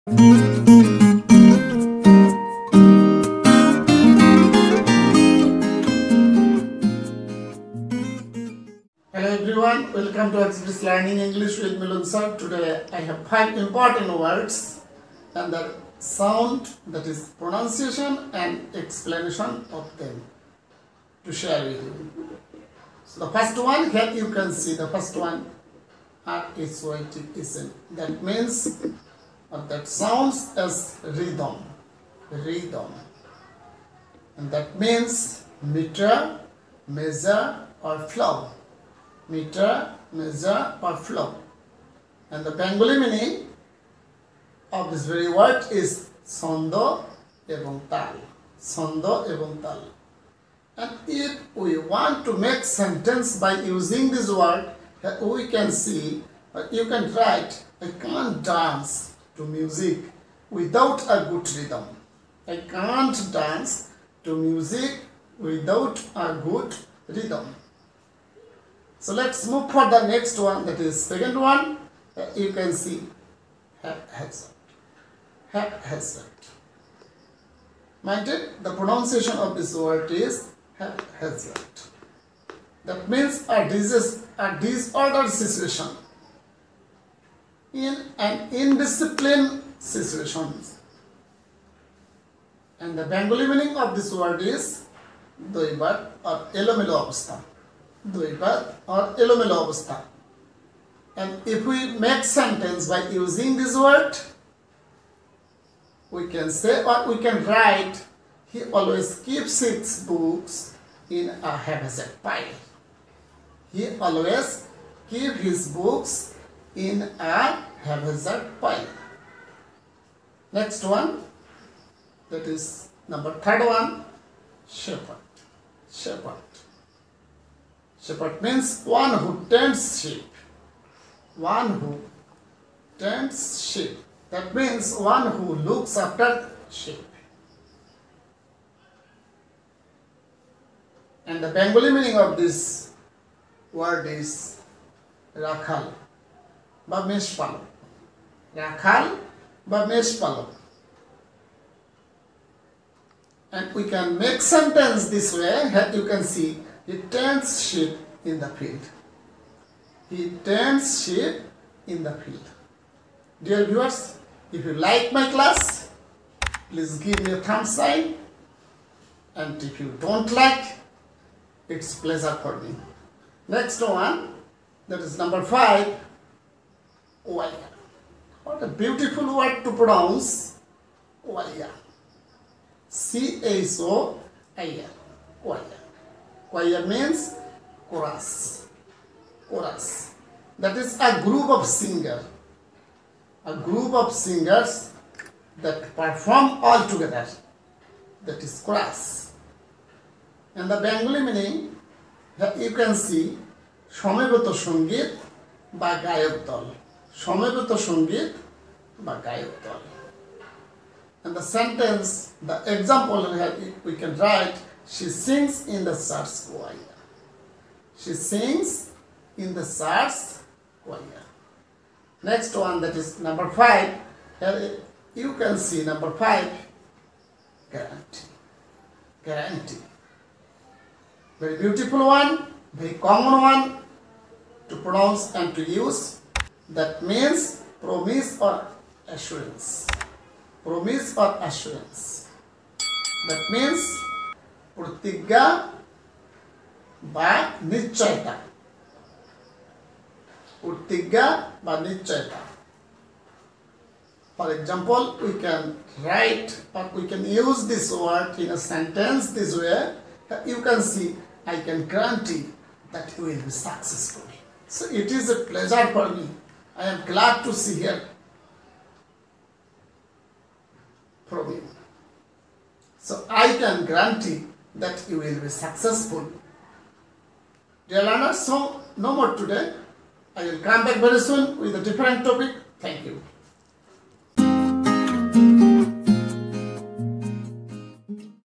Daily Use English Vocabulary|| English Words with Meaning|| English Words with Bengali Meaning
oxbridge _learningenglish
daily used Words
This video is about common English words that are really awesome. These types of words help us develop our daily life Vocabulary. oxbridge_learningenglish is always with you. So, let's get started.